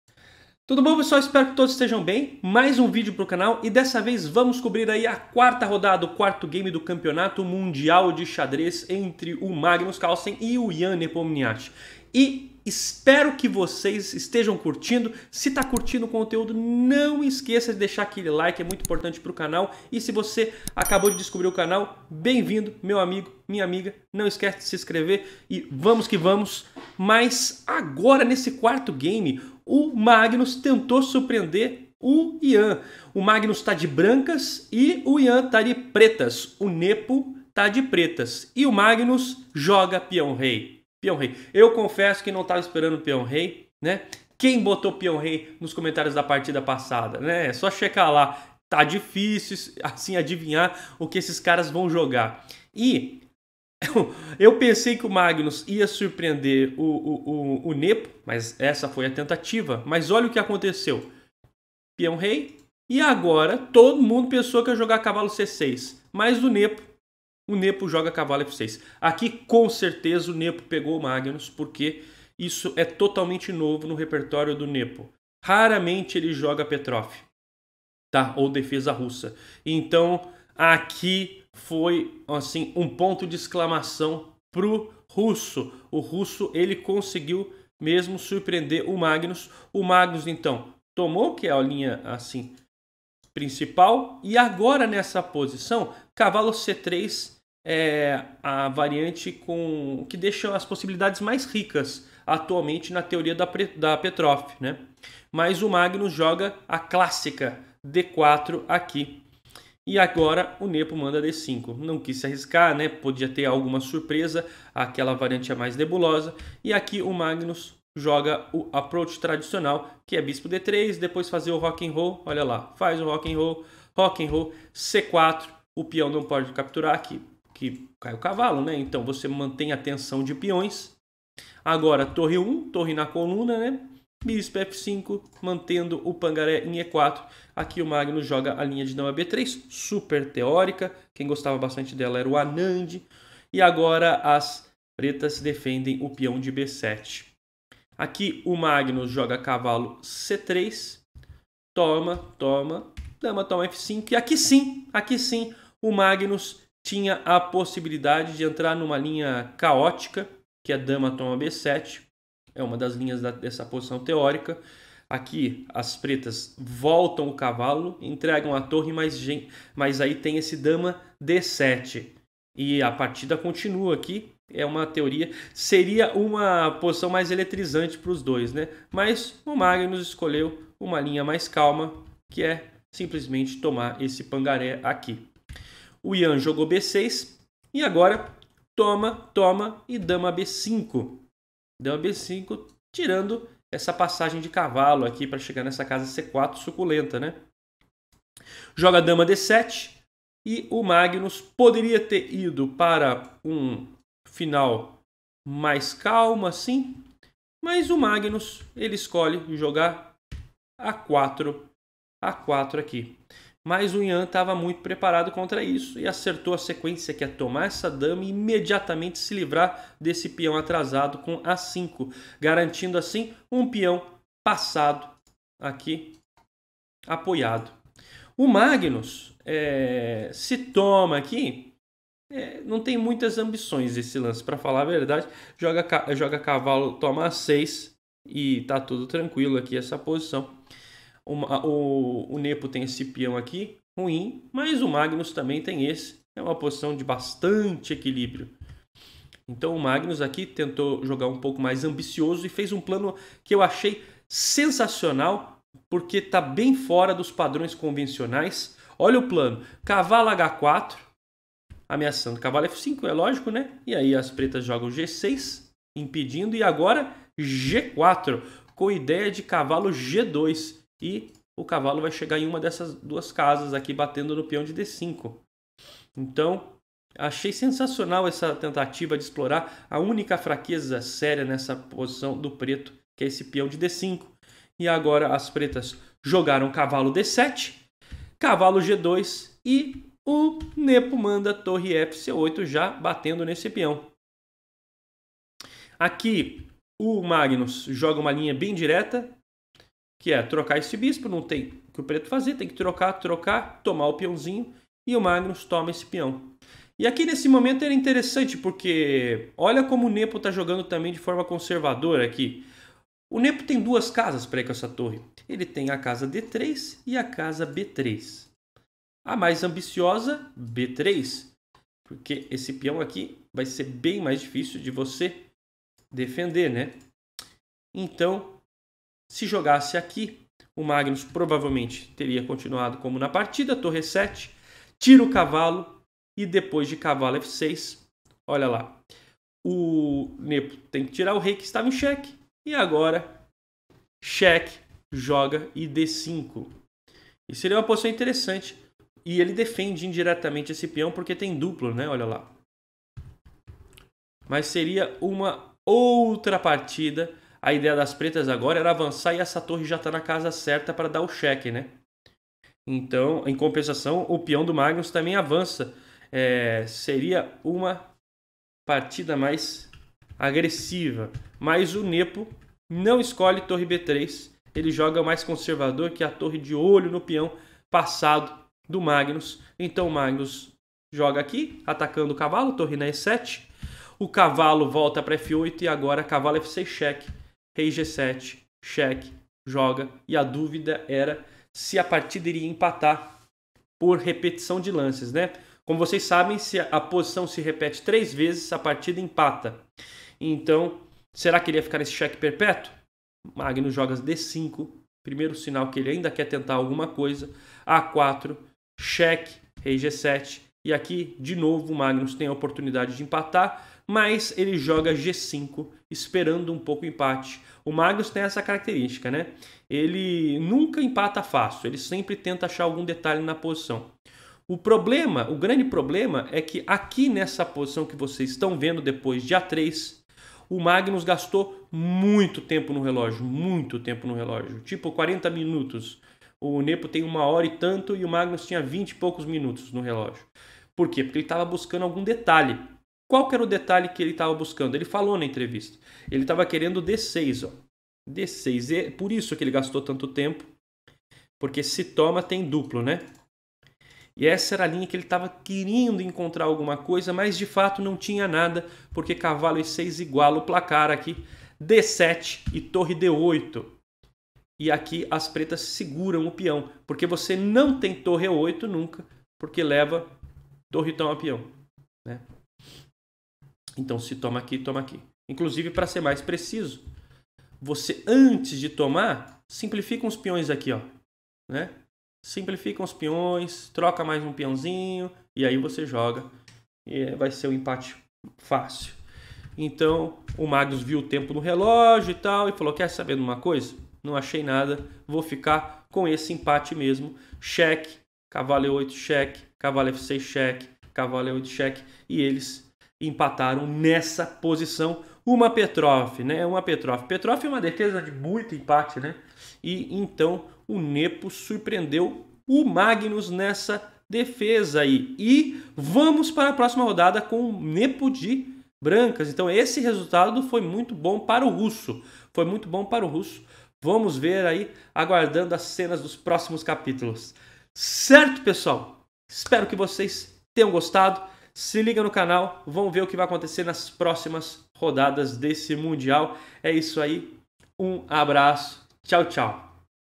Surpresa na abertura do russo Nepo x Magnus Carlsen na 4a rodada do mundial de xadrez
Campeonato Mundial 4 Rodada Carlsen x Nepo
Acompanhe os comentários da primeira partida do campeonato mundial de xadrez entre o Nepomniachti e Magnus Carlsen.
Uma partida interessante disputada pelo campeonato mundial de xadrez entre Magnus Carlsen e Ian Nepomniachti. Numa abertura russa ou petroff o russo Ian Neponiachti tenta abrir o placar contra Magnus Carlsen na quarta rodada do campeonato mundial de xadrez.